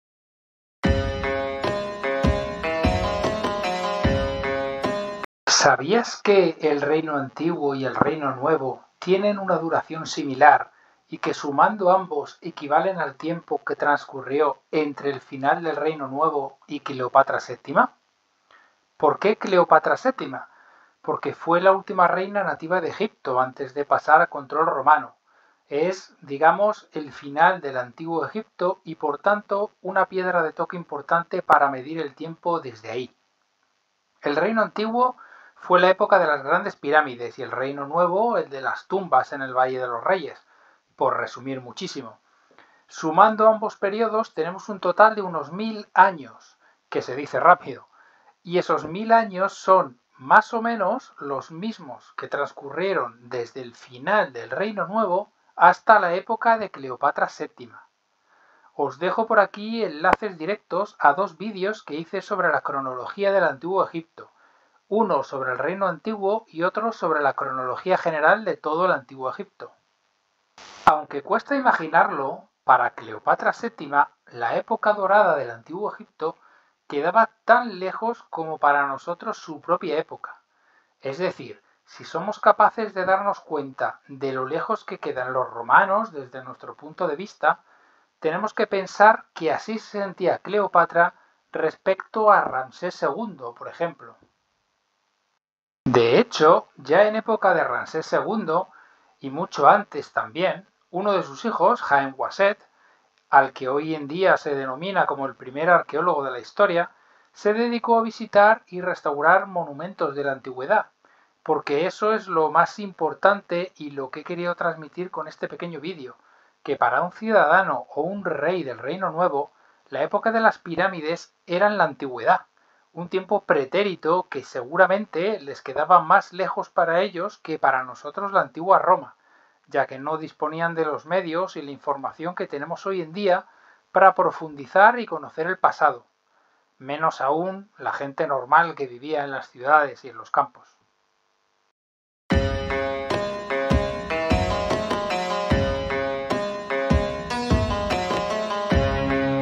¿Sabías que... en el antiguo Egipto: Perspectiva histórica
¿Alguna vez te has parado a pensar en que para los mismos egipcios la época de las pirámides quedaba tan lejos como para nosotros los romanos?